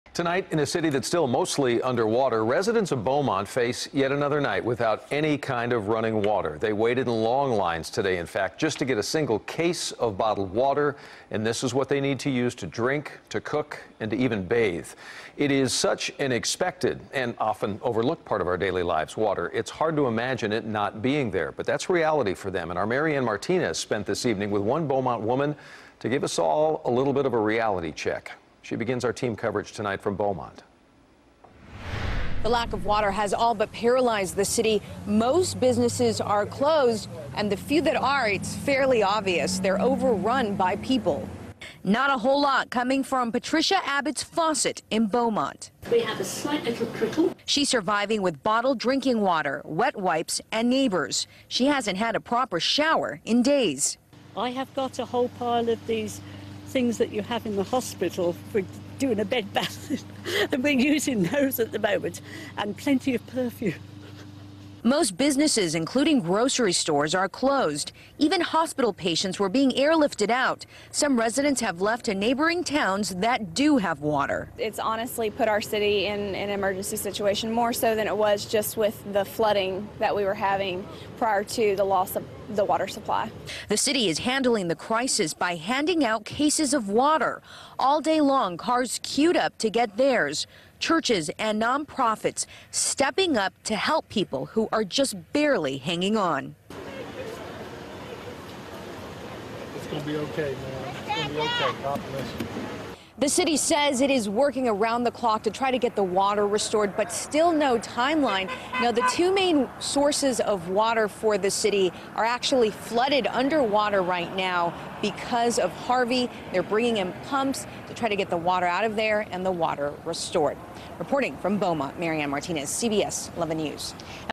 Two Days Later, Beaumont Still Without Water
It's a crisis worse than Harvey in one Texas city.